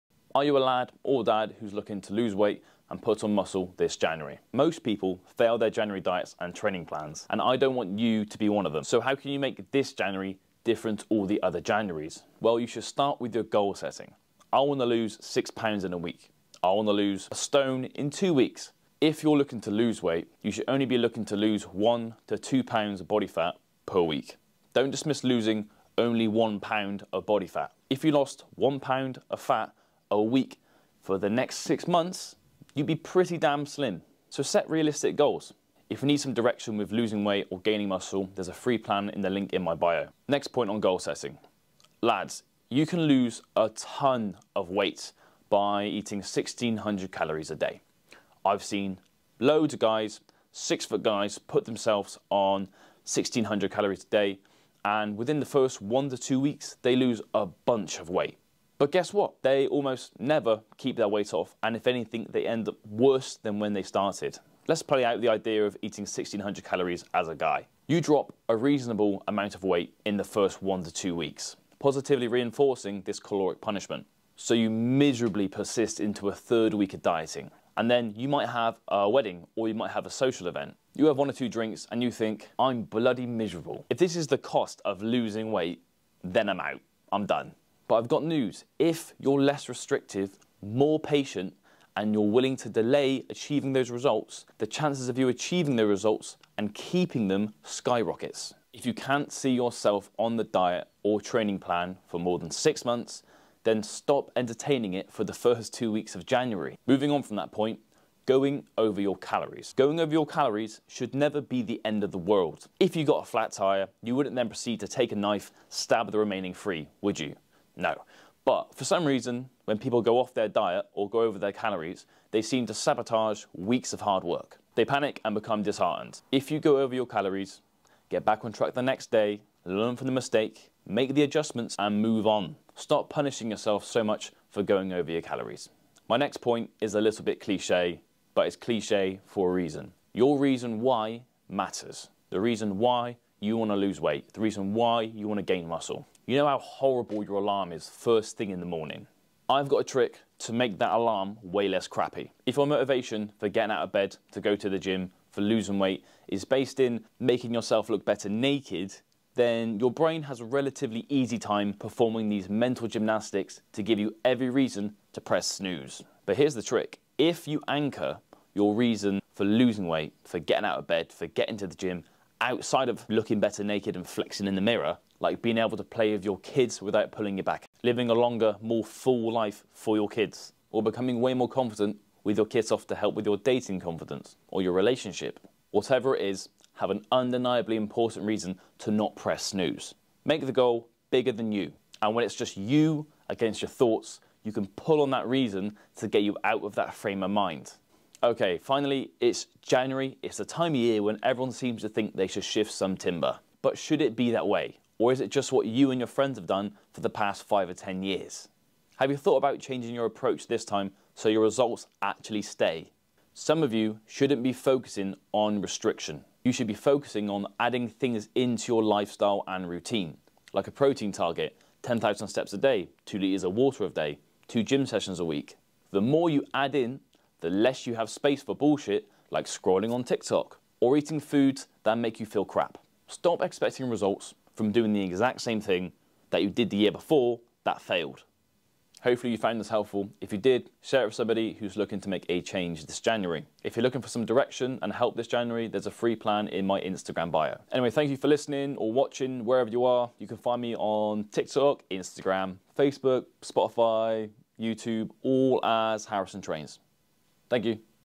HOW TO DIET IN JANUARY 2023!
HOW TO MAKE THIS JANUARY THE LAST TIME YOU NEED TO MAKE A CHANGE

In this video i cover some of the most important tips for losing weight, gaining muscle and making a change this January